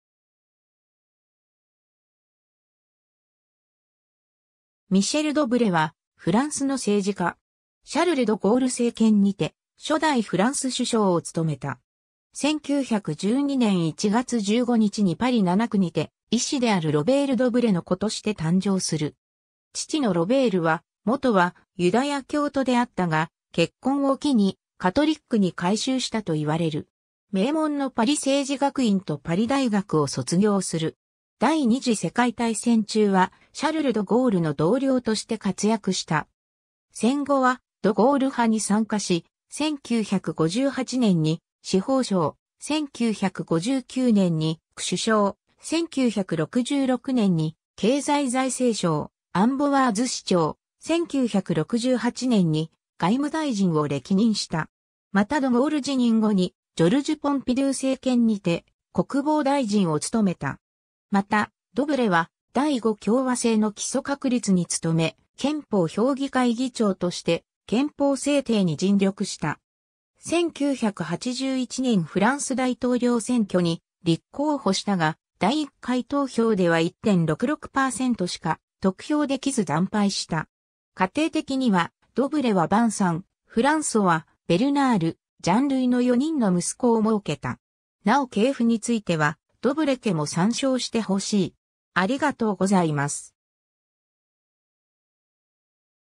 ミシェル・ドブレ
アカデミー・フランセーズ会員
ミシェル・ドブレ（フランス語：Michel Debré、1912年1月15日 – 1996年8月2日）は、フランスの政治家。
シャルル・ド・ゴール政権にて初代フランス首相を務めた。
 1912年1月15日にパリ7区にて、医師であるロベール・ドブレの子として誕生する。
父のロベールは元はユダヤ教徒であったが、結婚を機にカトリックに改宗したと言われる。
名門のパリ政治学院とパリ大学を卒業する。
 第二次世界大戦中はシャルル・ド・ゴールの同僚として活躍した。
戦後はド・ゴール派に参加し、1958年に司法相、1959年に首相、1966年に経済財政相（蔵相）、アンボワーズ市長（1989年まで）、1968年に外務大臣を歴任した。
またド・ゴール辞任後にジョルジュ・ポンピドゥー政権にて国防大臣を務めた。
 また、ドブレは第五共和政の基礎確立に努め、憲法評議会議長として憲法（1958年憲法と呼ばれる）制定に尽力した。
 1981年フランス大統領選挙に立候補したが、第1回投票では1.66パーセントしか得票できず惨敗した。
 家庭的には、ドブレはヴァンサン、フランソワ、ベルナール、ジャン・ルイの4人の息子を儲けた。
なお系譜については、ドブレ家も参照して欲しい。